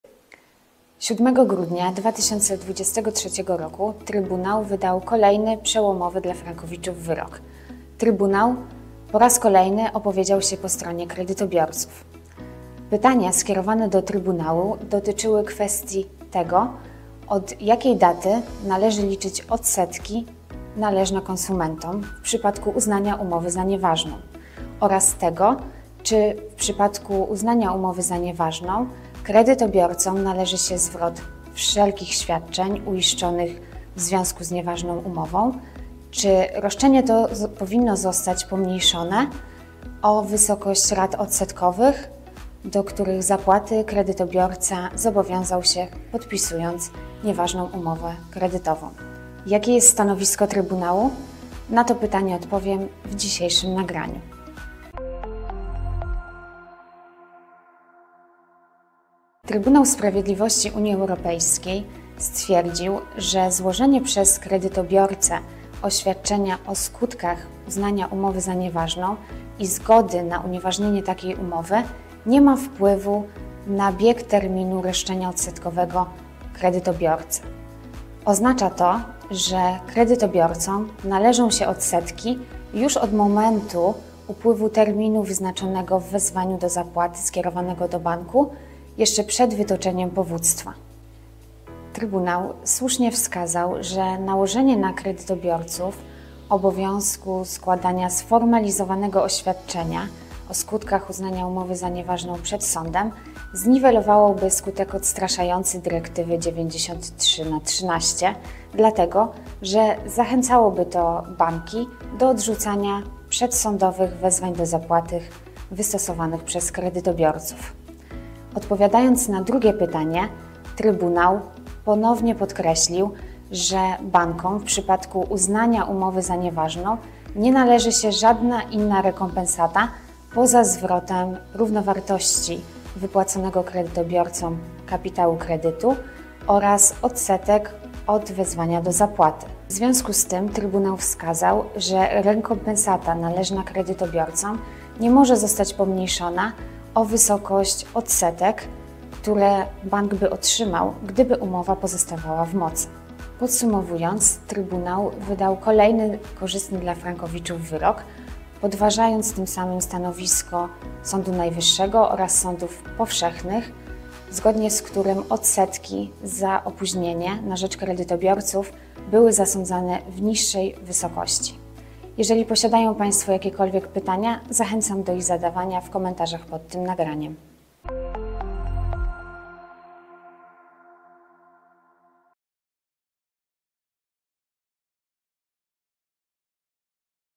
TSUE kasuje oświadczenia frankowiczów - wyrok C-140/22
Wczoraj TSUE wydał długowyczekiwany wyrok w polskiej sprawie C-140/22, w którym ponownie stanął po stronie frankowiczów!

Odpowiedział w nim na bardzo ważne kwestie:

👉 od kiedy należy liczyć odsetki należne konsumentom w przypadku uznania umowy za nieważną?
👉 czy kredytobiorca musi składać przed sądem oświadczenie o skutkach ustalenia nieważności umowy kredytu, aby sąd unieważnił taką umowę?
👉 czy roszczenie kredytobiorcy powinno zostać pomniejszone o wysokość rat odsetkowych, do których zapłaty kredytobiorca zobowiązał się podpisując nieważną umowę kredytową?

Wyrok skomentowała adw. Karolina Kwiatkowska - partner naszej Kancelarii.

Zapraszamy do zapoznania się z całością nagrania, a w przypadku pytań do pozostawienia ich w komentarzach pod tym postem. 🗯

Jeżeli spodobało Ci się to nagranie - zapoznaj się z innymi filmami na naszej playliście Poradnik Frankowicza: